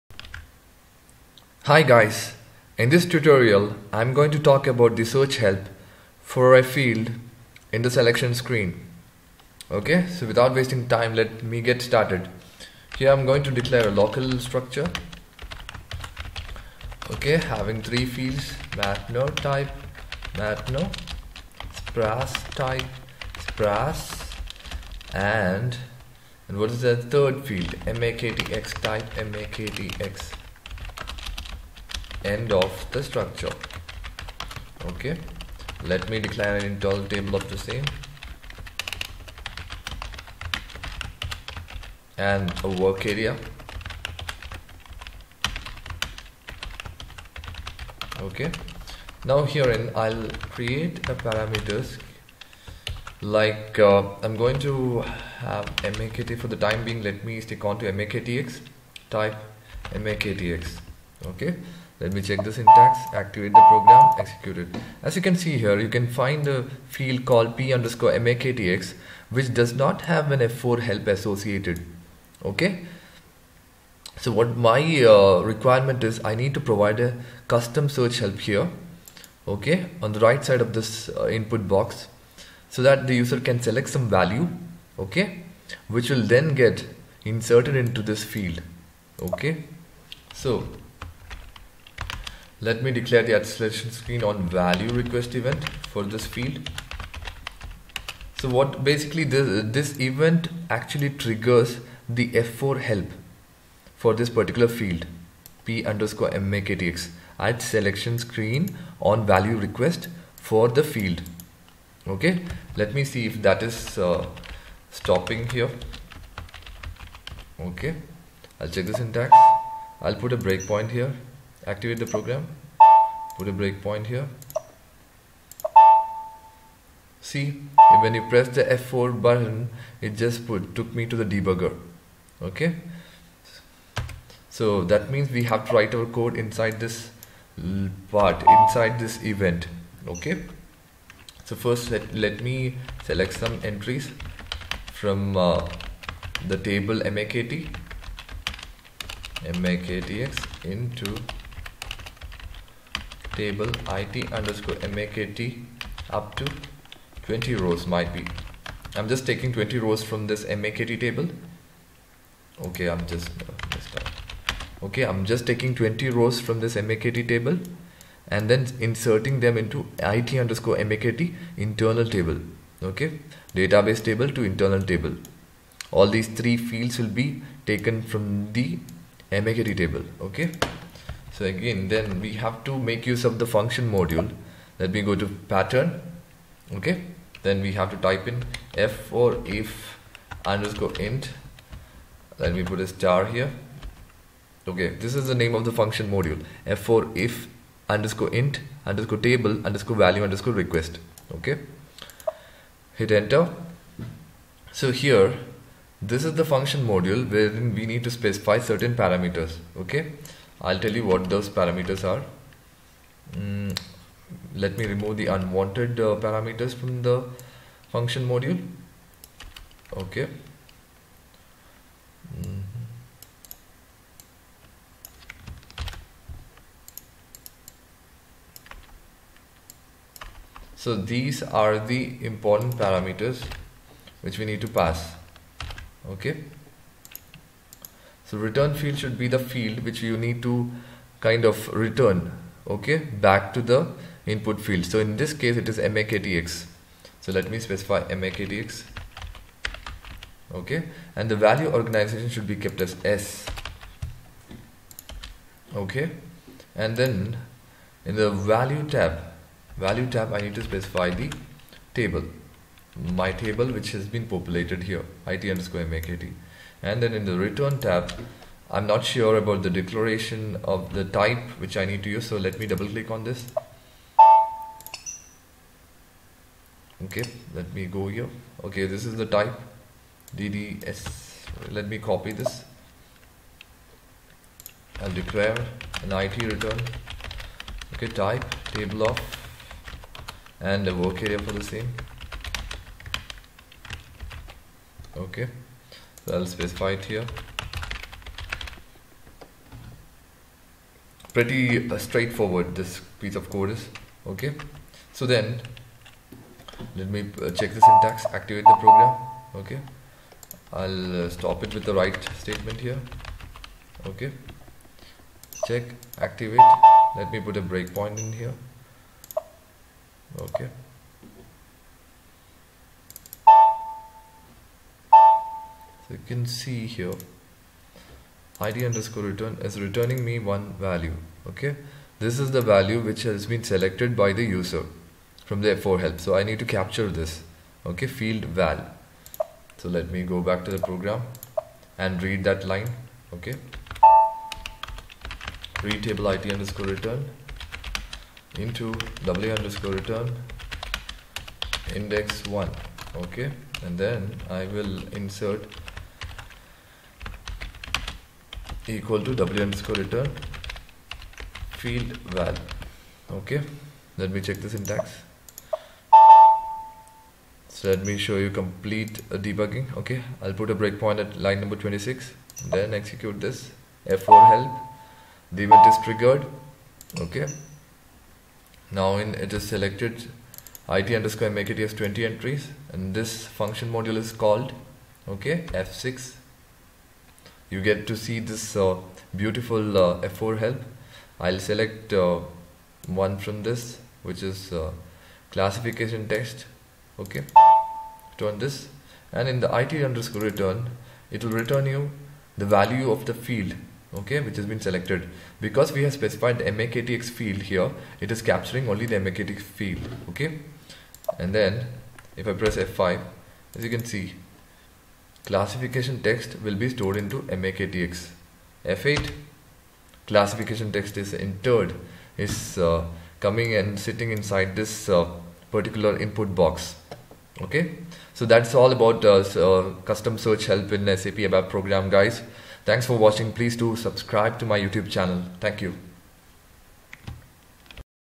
SAP ABAP : F4 Help in Selection Screen 《2019》
SAP ABAP : F4 Help Selection Screen by Sooraj-2017 Tutorial
In this tutorial, I will be discussing on how to create f4 help for a field on the selection screen in abap.
This is a very exciting topic as this would be required in many real time cases when working in a project.